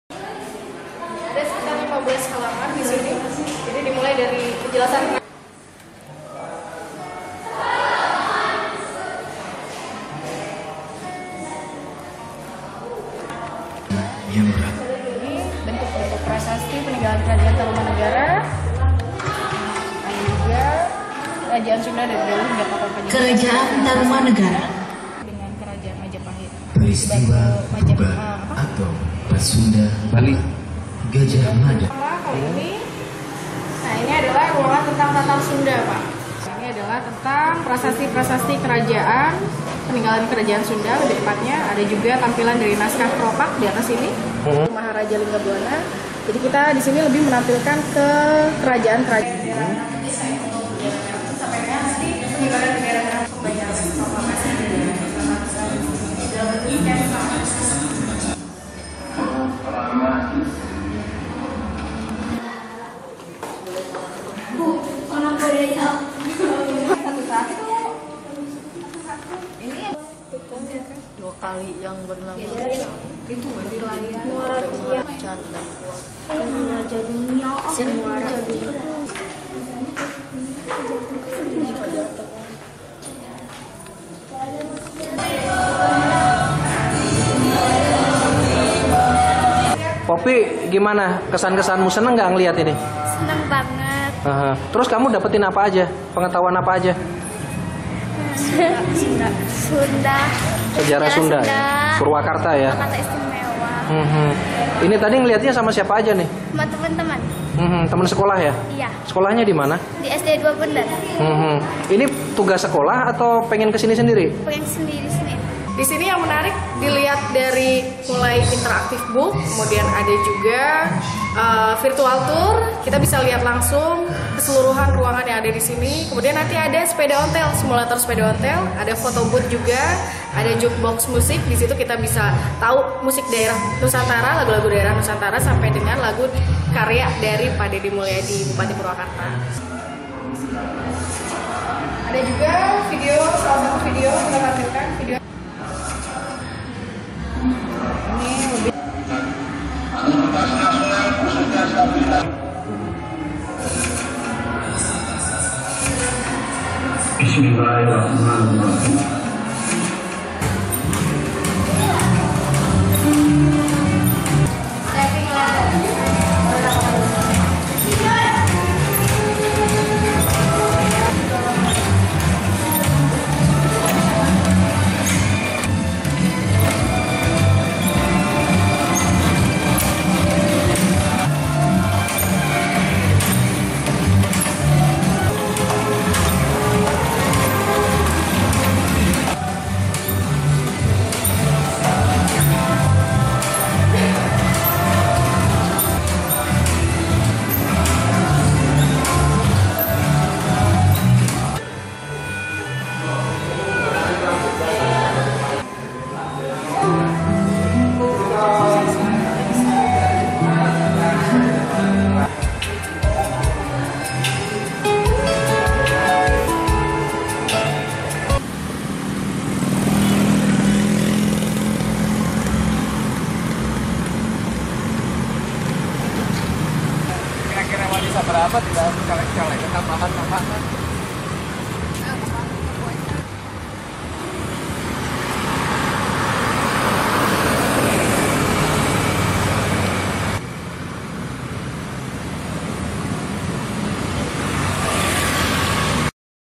Menikmati Sejarah Secara Virtual di Purwakarta
Bale Panyawangan Purwakarta
Jalan K.K Singawinata Purwakarta

Sejarah memang bisa dikenalkan dengan berbagai cara. Di Kabupaten Purwakarta, Jawa Barat misalnya yang memiliki sebuah tempat dengan teknik memadukan data sejarah dan teknologi. Dinamakan Balai Panyawangan, tempat memiliki berbagai informasi sejarah Sunda dan eksistensi Purwakarta bagi bangsa yang disajikan dalam buku besar bisa dioperasikan hanya dengan isyarat tangan.  Berbagai informasi sejarah pun dapat dinikmati secara mudah dan menyenangkan.